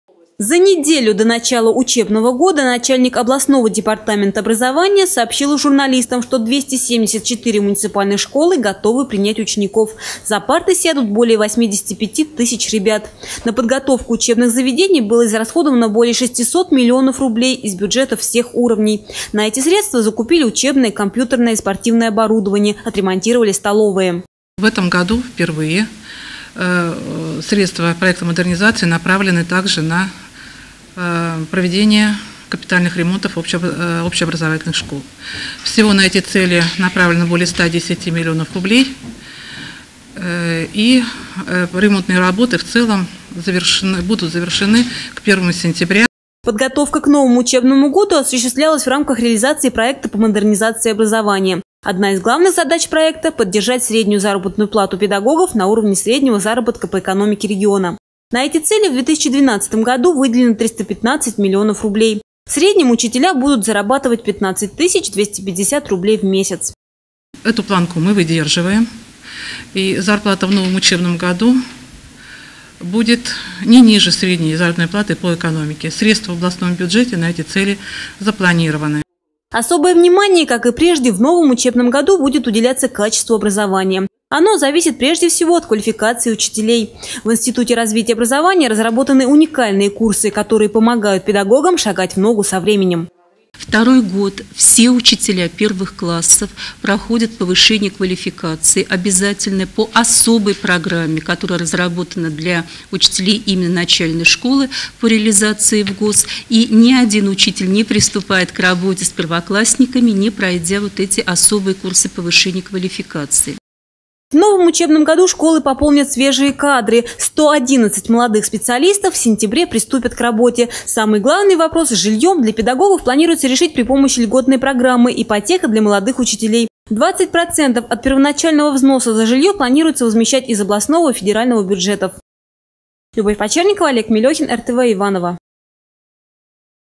Льготная ипотека для молодых педагогов
В новом учебном году школы пополнят свежие кадры. 111 молодых специалистов в сентябре приступят к работе.